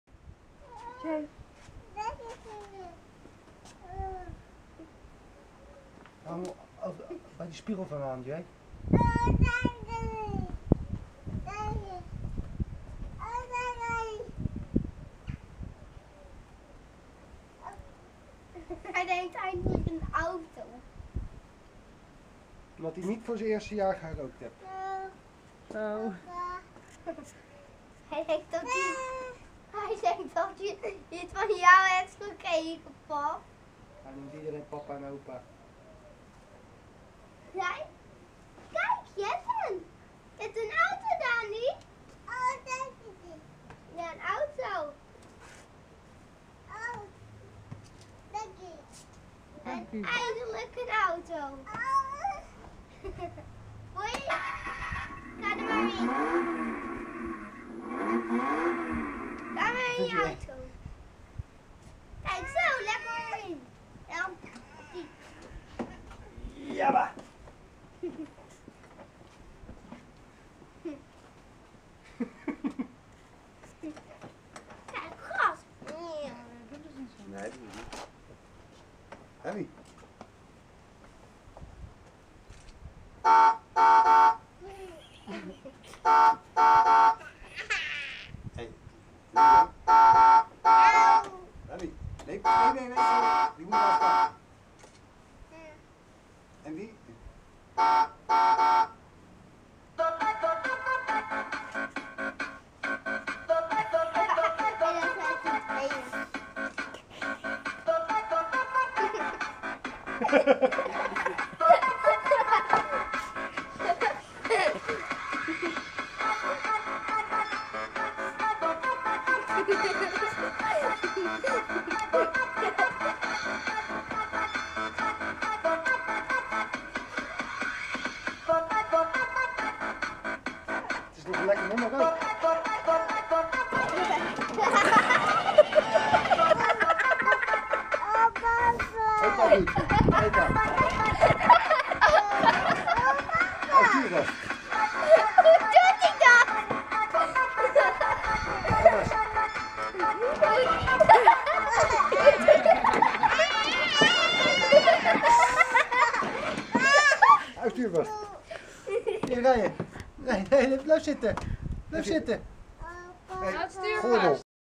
Accuautocar Maserati
Dani zijn nieuwe Maserati van accuautoshop.nl. Hij vindt het prachtig  al die lichtjes en muziekjes. Alleen het rijden vindt hij nog een beetje eng. Maar hij is 21 mnd. Dus alles nog een beetje verkennen. maar het gaat goed komen. Als er maar muziek in zit vindt hij helemaal geweldig.